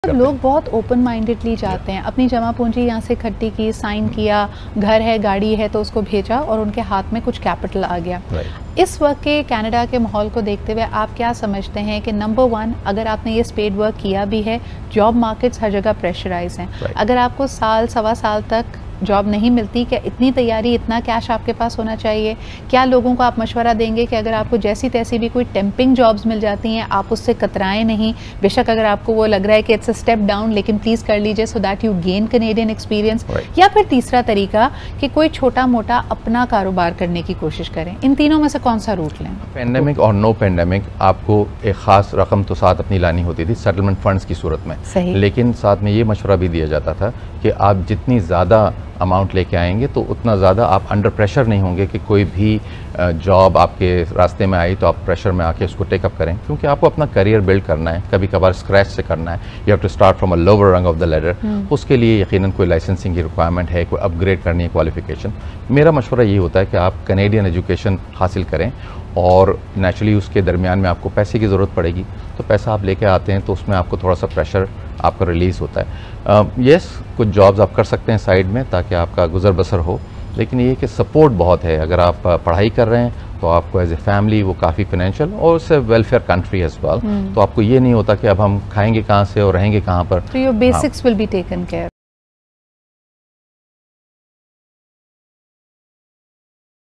How much money to bring to Canada? Amir Ismail on AAJ TV
Immigration to Canada from Pakistan in 2022. How much funds are required to bring with you to Canada. Toronto-based licensed immigration consultant Amir Ismail on AAJ TV